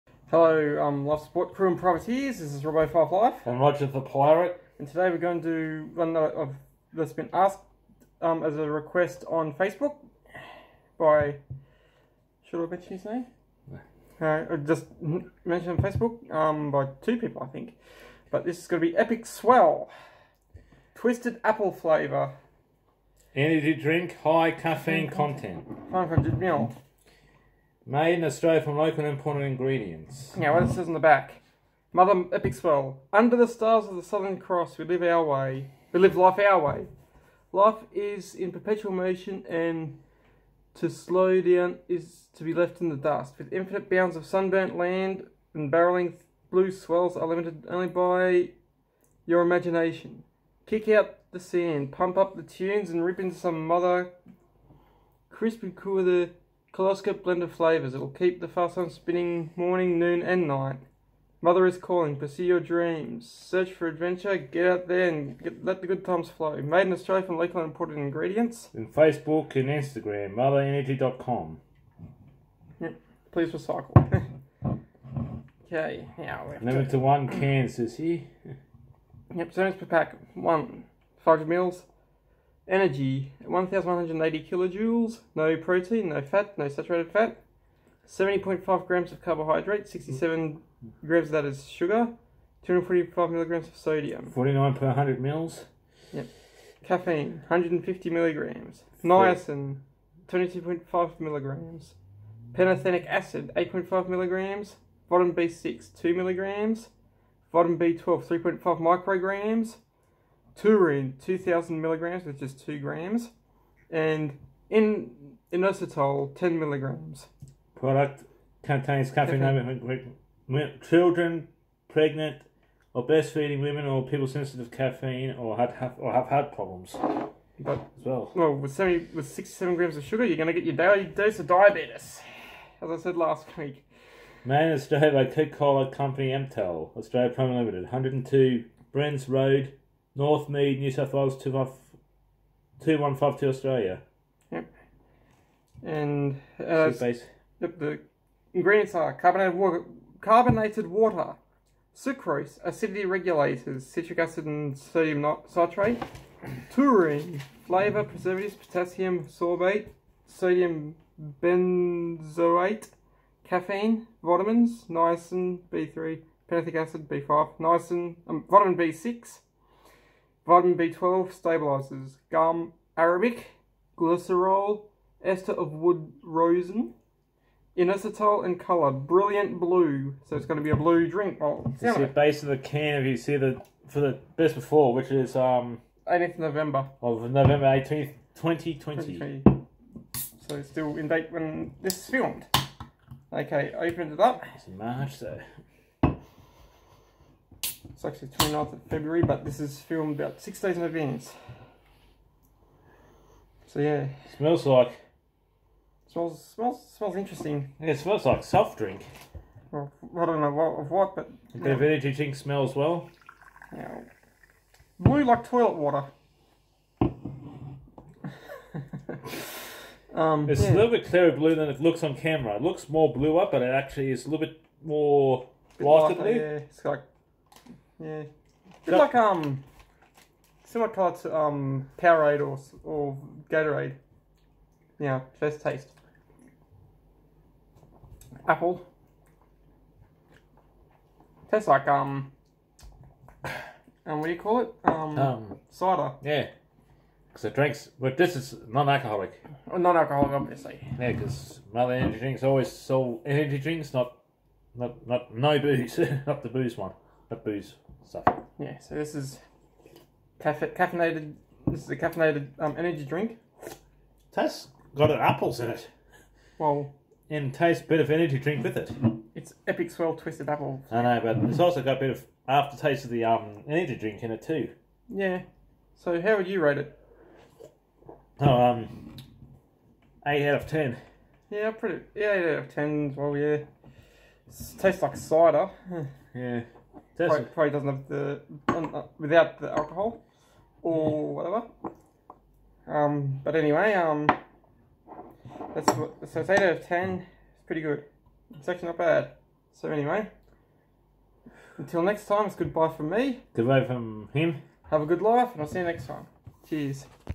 Mother Epic Swell Twisted Apple Flavor By Roger Th Pirate FT Robbo5 Life
A Joint Review  of Mother Epic Swell Twisted Apple Flavor by Roger Th Pirate FT Robbo 5 Life